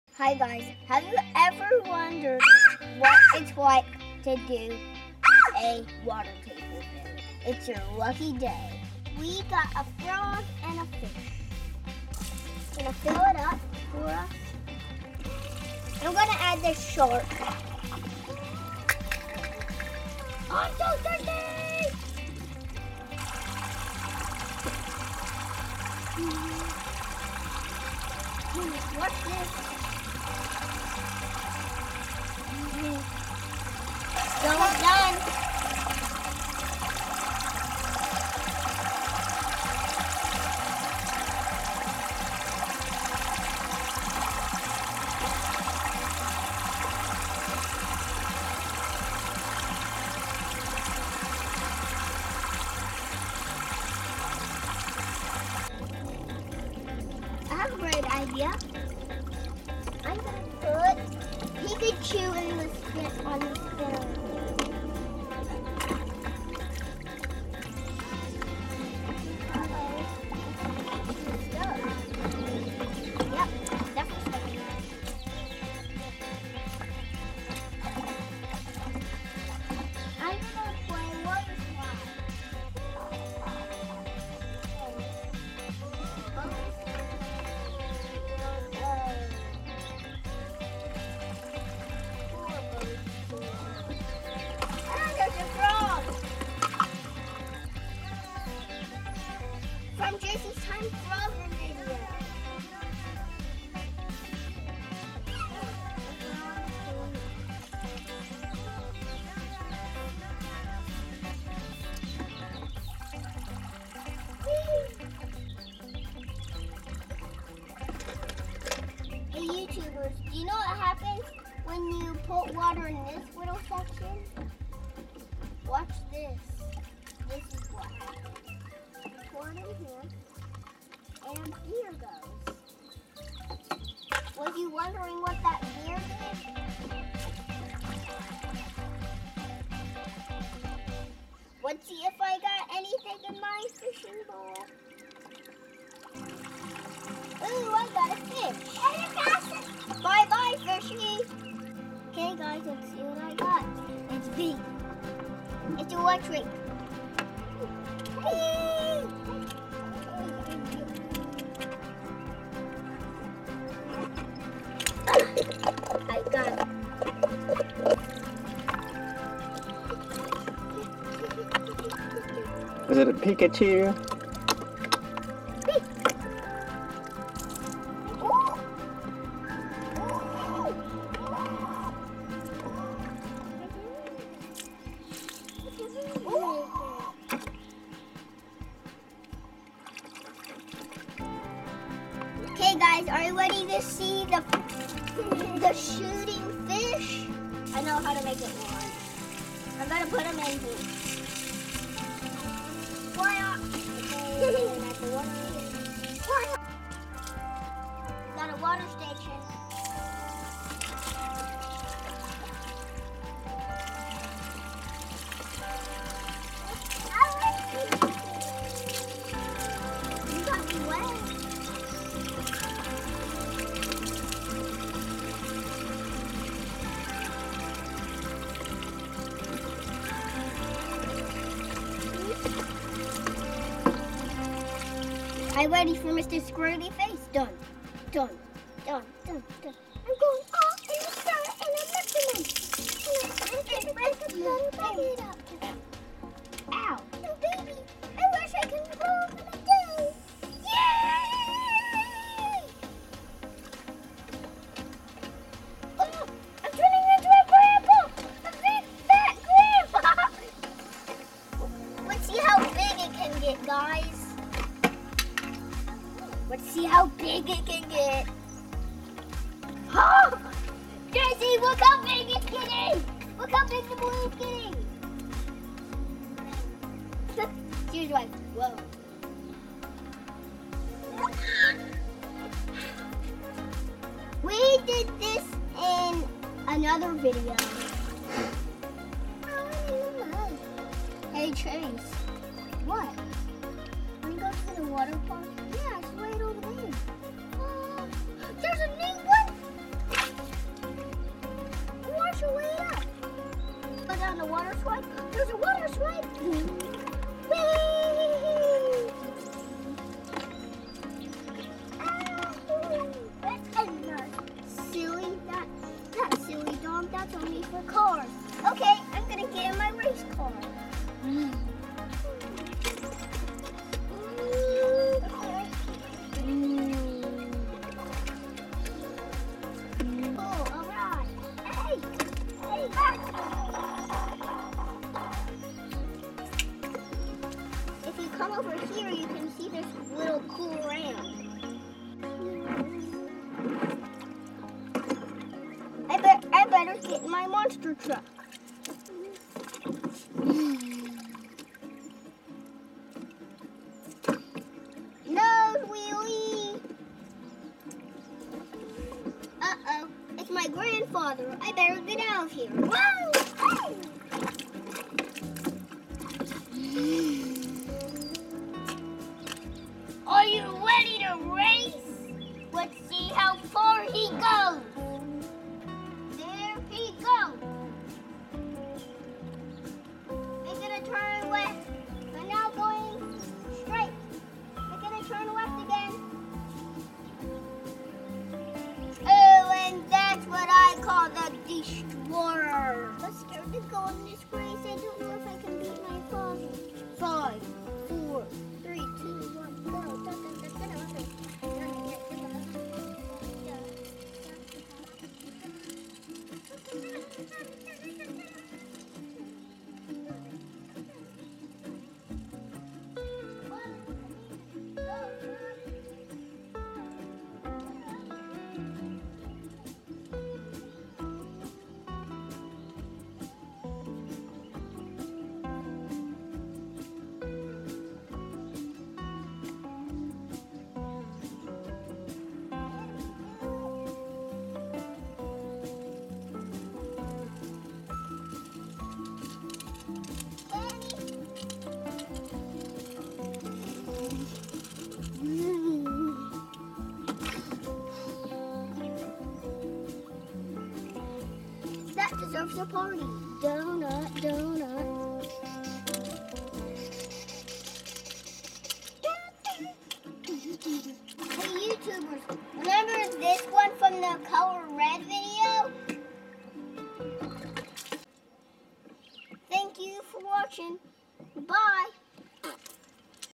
Double Showers Water Table Step 2 Outside Playtime
Today we play with the Double Showers Water Table.

We will have Suprise toys in this video. Let's see who comes by